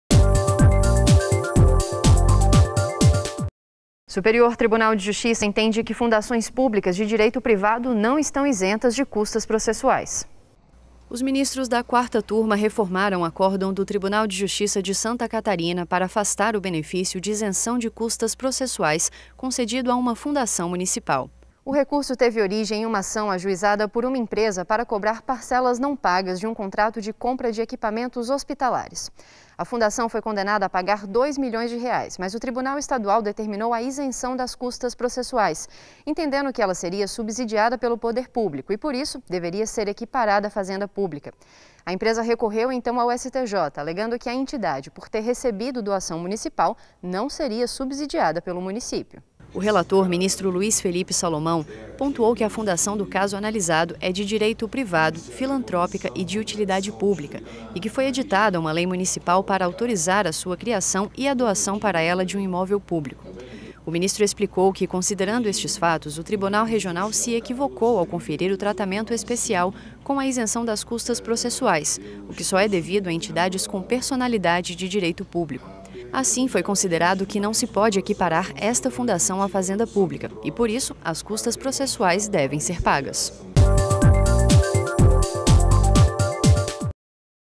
Fundações públicas de direito privado não estão isentas de custas processuais 16.09.20
​As fundações públicas de direito privado, cuja criação é autorizada por lei, não são equiparadas à Fazenda Pública e não fazem jus a isenção de custas processuais. Com esse entendimento, a Quarta Turma do Superior Tribunal de Justiça (STJ) reformou acórdão do Tribunal de Justiça de Santa Catarina (TJSC) para afastar o benefício concedido a uma fundação municipal condenada por descumprimento contratual.
REsp 1409199